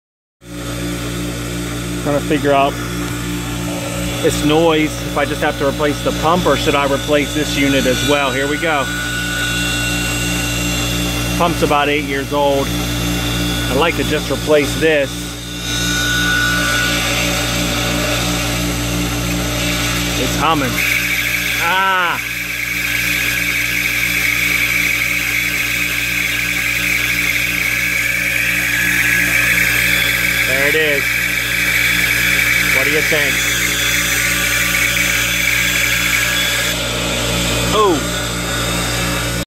Pool Pump failing. 1hp Hayward. Thoughts?
What do i Need to buy? Just the pump or the pump and the water container that it connects to?